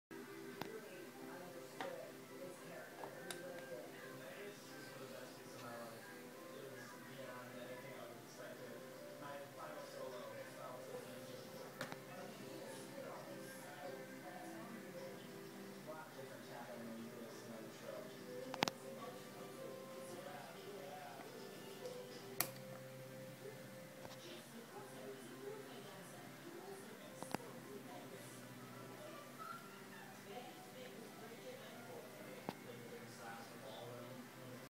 Han in Carbonite HiC lighting kit
Complete screen accurate Han in Carbonite lighting kit for your hero panel. This one has an additional power switch wired in, otherwise you can use the included rotary knob to turn the lights on/off and switch between the slow frozen flash pattern and the fast unfreeze flash pattern.